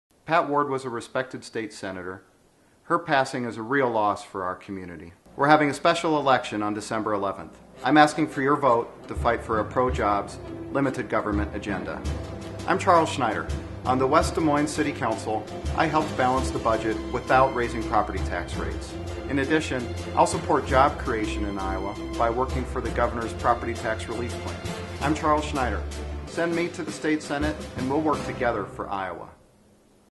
Charles Schneider for State Senate
Charles Schneider is the republican nominee for the Senate District 22 Special Election.   The election is December 11, 2012.  The district inclues Clive, Waukee, West Des Moines and Windsor Heights in Iowa.